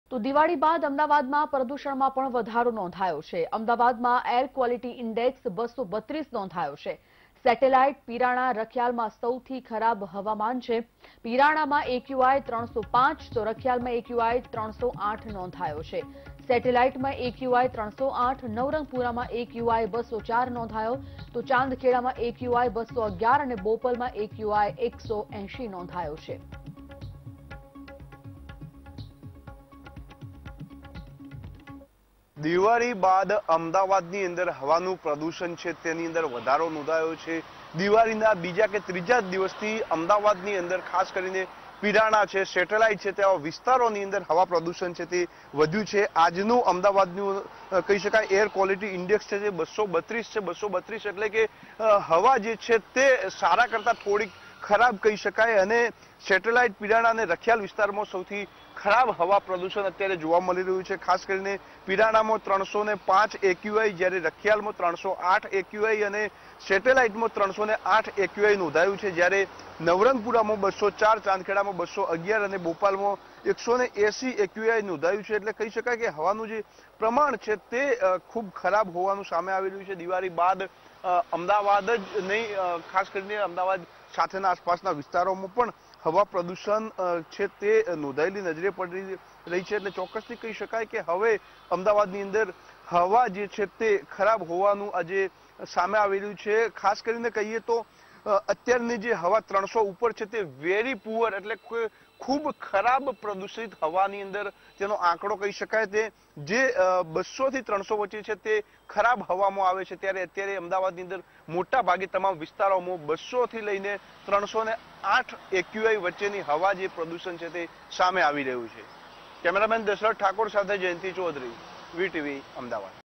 Diwali બાદ Ahmedabad માં પ્રદૂષણમાં વધારો, જાણો કયા વિસ્તારમાં સ્થિતિ સૌથી ખરાબ | VTV Gujarati
Diwali બાદ Ahmedabad માં પ્રદૂષણમાં વધારો, જાણો કયા વિસ્તારમાં સ્થિતિ સૌથી ખરાબ